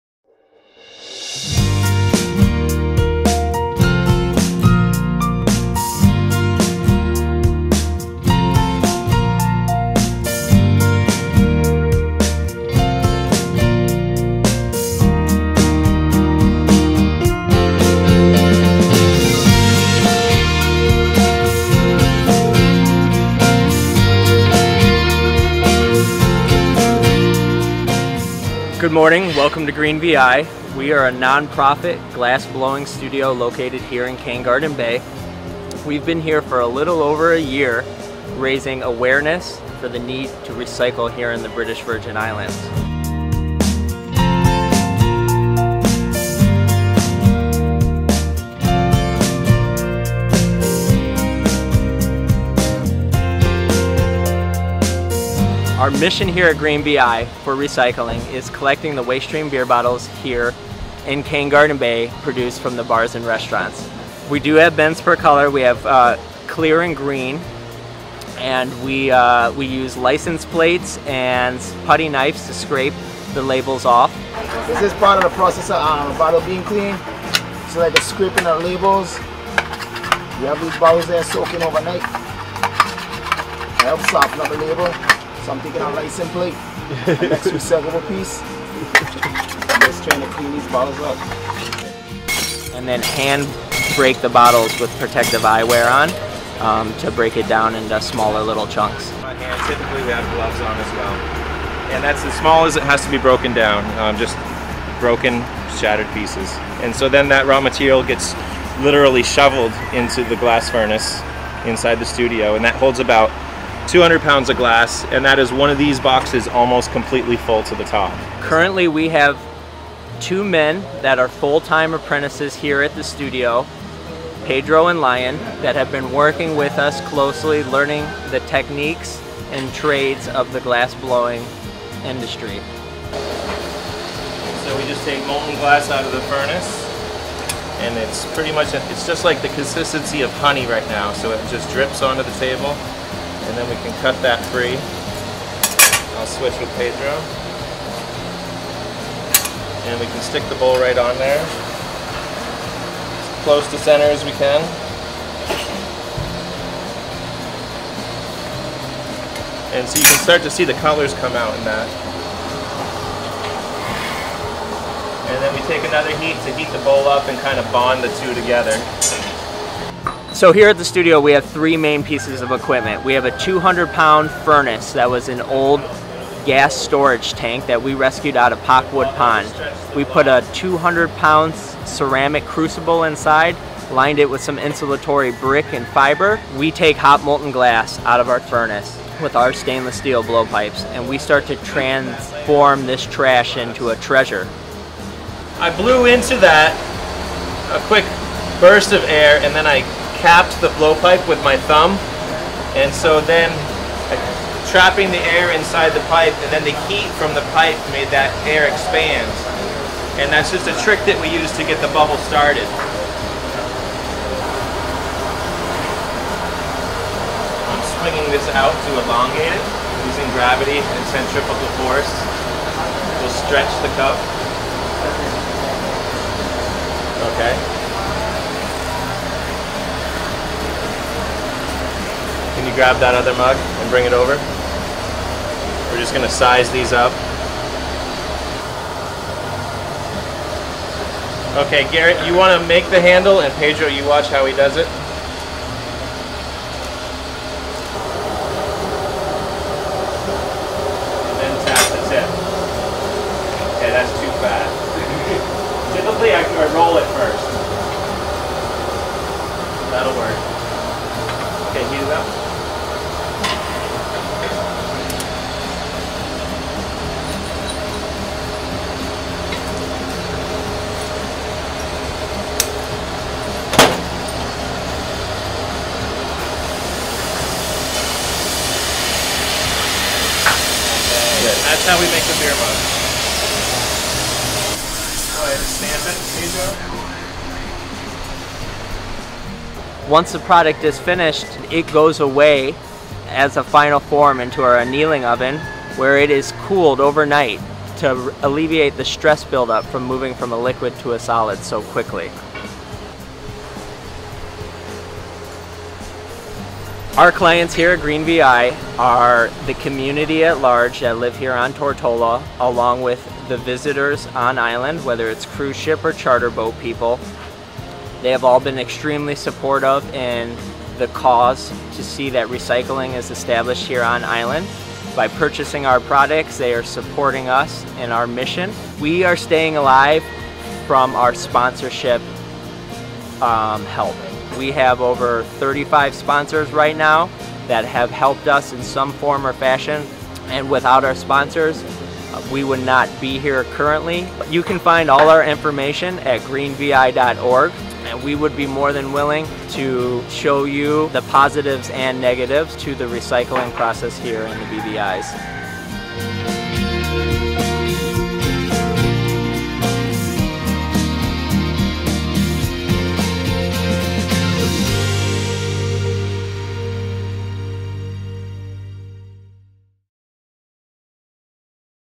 Making Glass Sculptures with GREEN VI in the British Virgin Islands, CARIBBEAN!
Take a tour with the glass artists at the new Green VI in Cane Garden Bay on Tortola in the BVI's... they gave us an up close and personal instruction on their glass recycling procedure where they melt down bottles and create beautiful sculptures and souvenirs for locals and tourists alike!  Green VI is also the first major recycling effort in the British Virgin Islands, so be sure to stop by and support them when in the area!